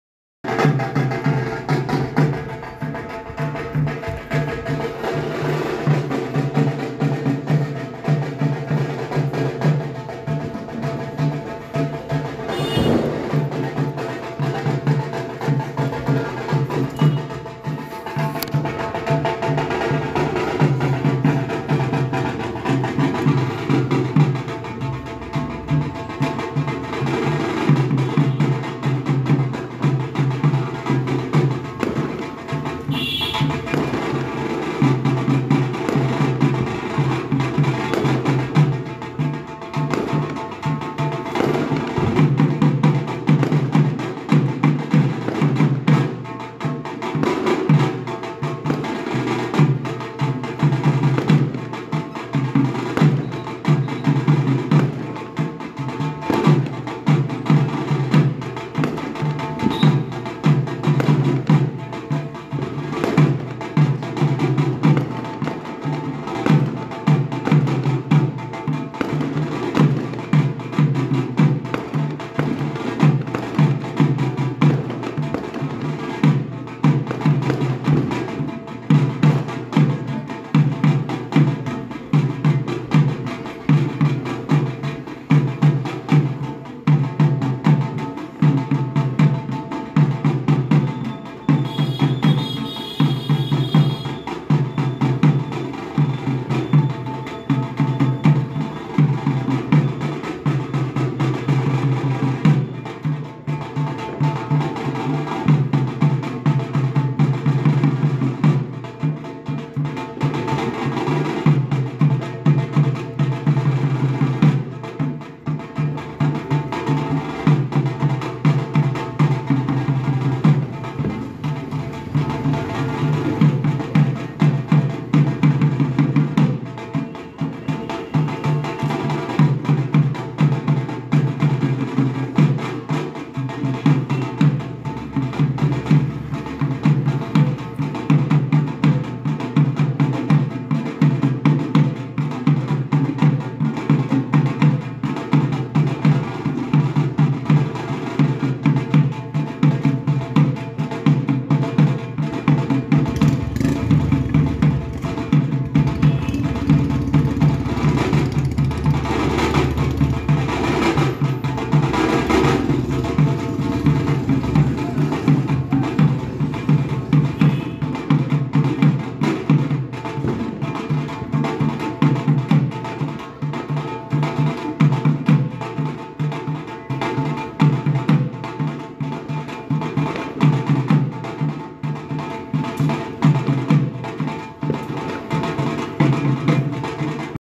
Vaishno Devi Temple now at Krishnanagar Nadia 🙏🪷🇮🇳
Friends aj apnara Krishnanagar er Vaishno Devi Temple darshan korun. apnader khub valo lagbe.